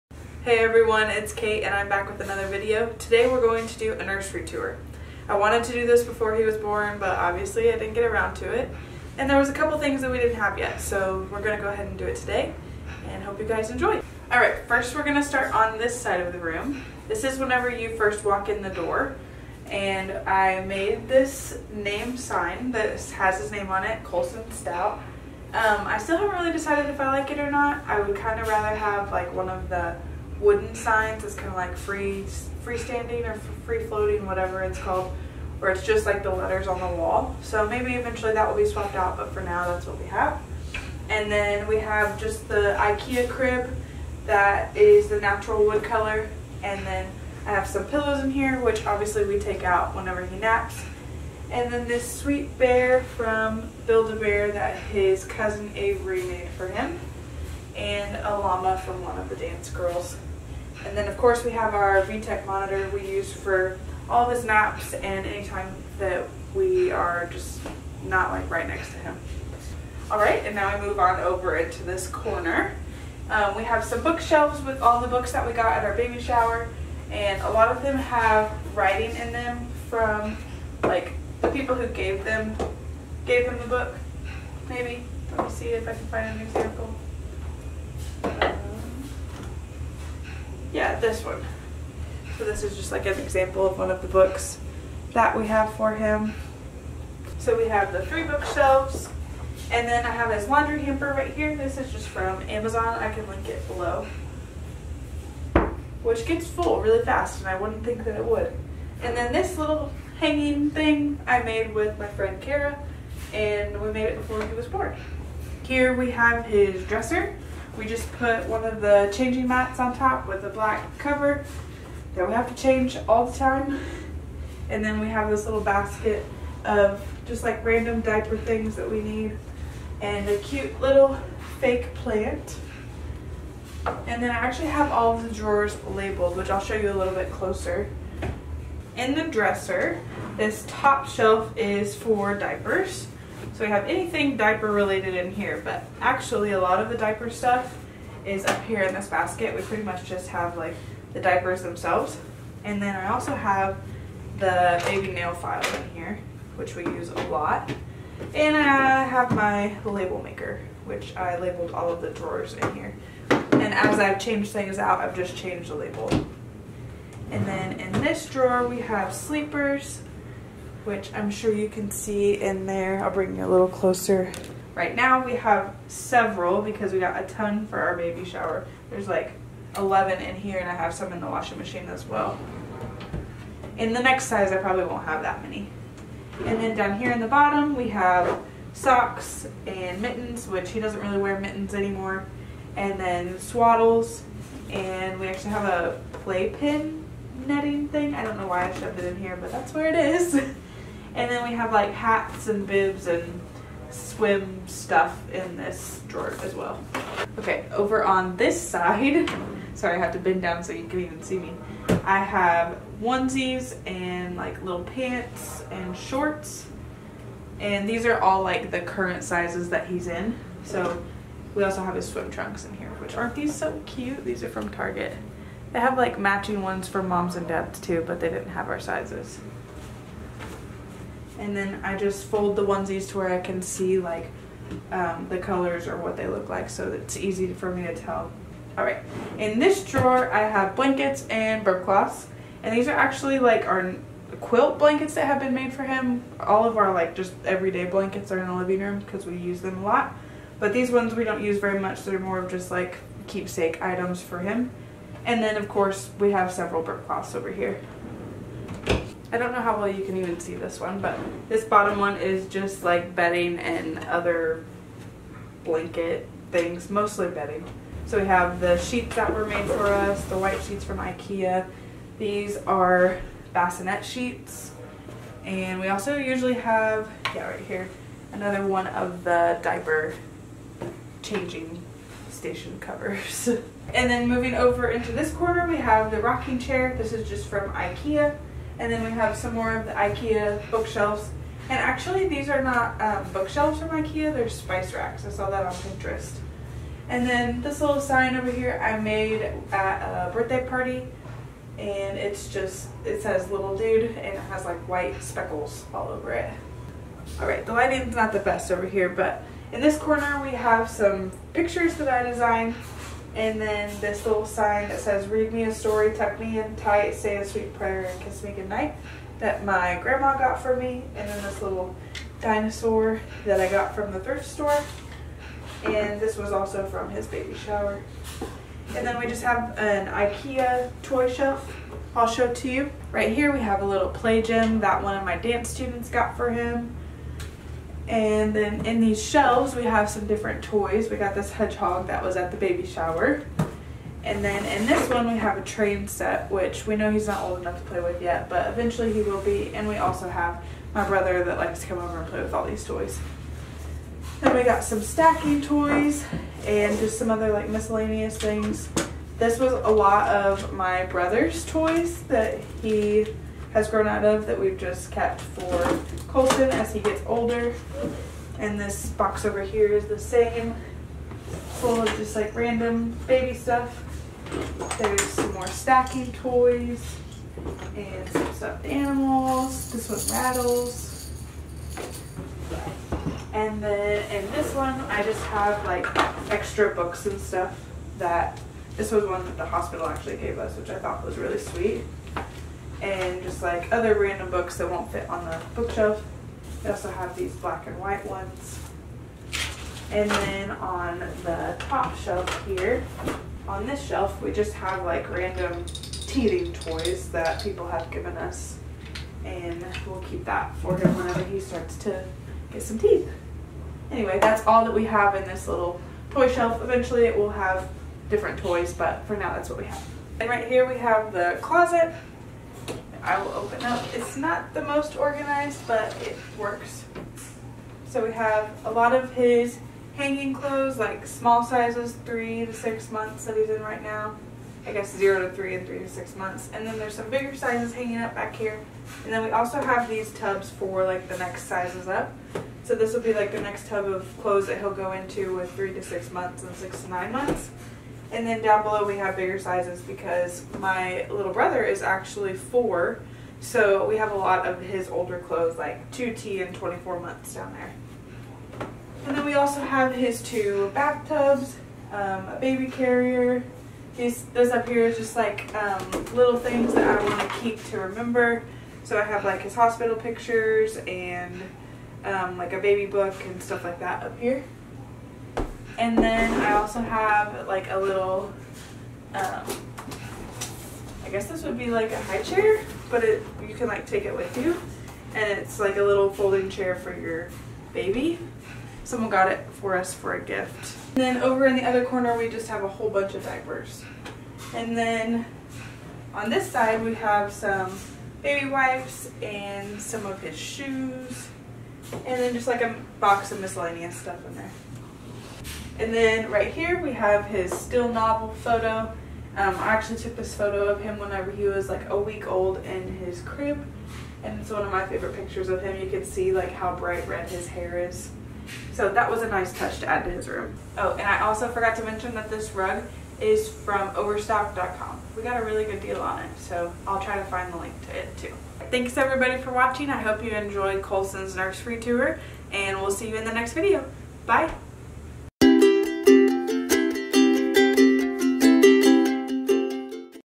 Kohlson's Nursery Tour
I have been meaning to make this video forever!  Just now getting around to it, but I kind of like that idea.  You can see how our nursery actually functions, we've changed a lot since before he was born!

Laundry Basket